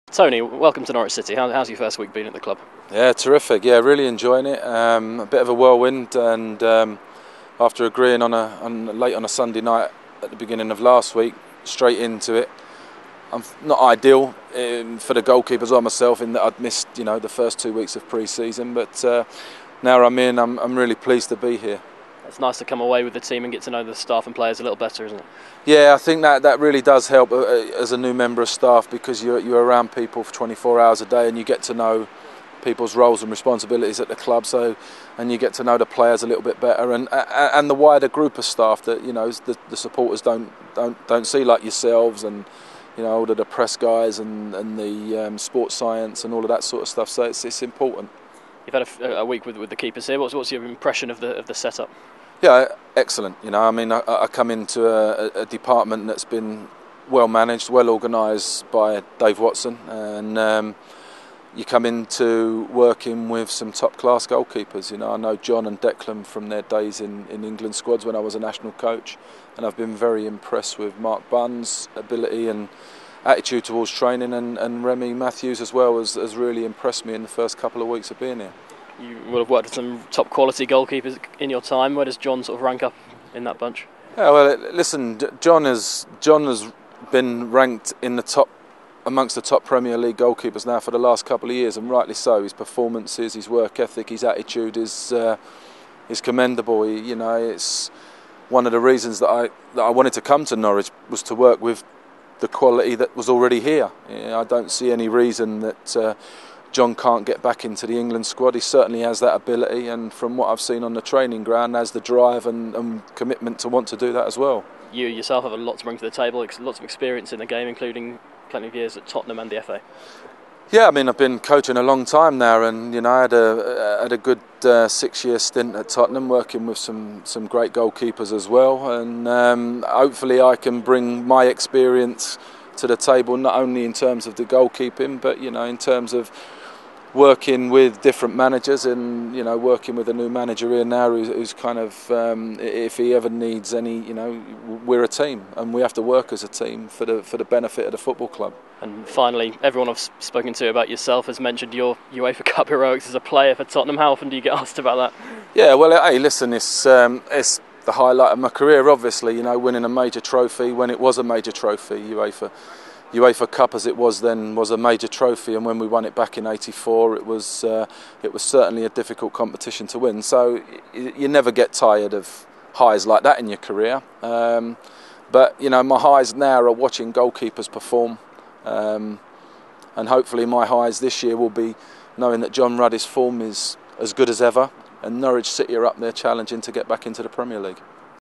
Tony Parks: Meet City's New GK Coach
Tony Parks speaks exclusively to Norwich City TV in his first interview since arriving at the club during the Canaries' pre-season tour to Italy.

Parks touches on:
- His move to Norwich after six seasons at Tottenham Hotspur
- His evaluation of the City goalkeepers
- His playing career at Spurs, including the 1984 UEFA Cup triumph